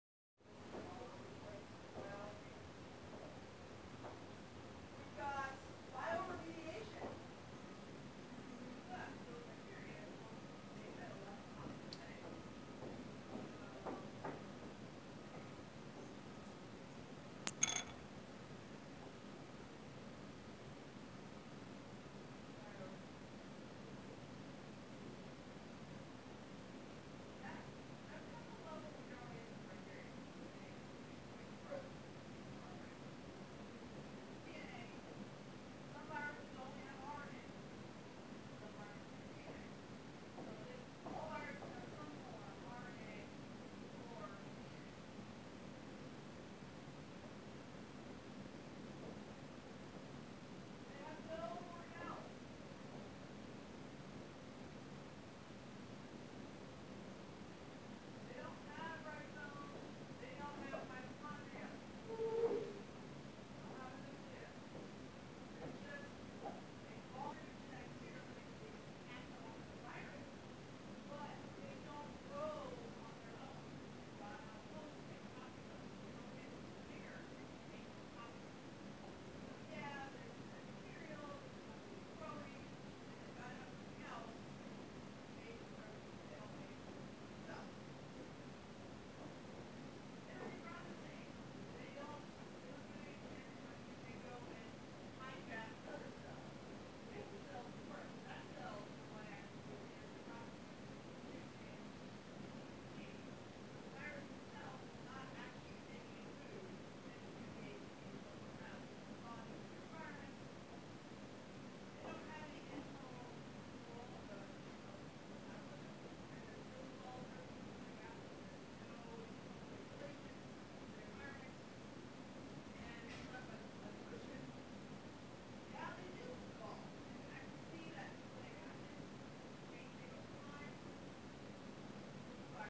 Brownian Movement: Carmine powder under compound microscope
You will notice large particles suspended in water in motion and relate the motion observed to forces that bring about diffusion. What happens to the larger particles compared to the smaller particles?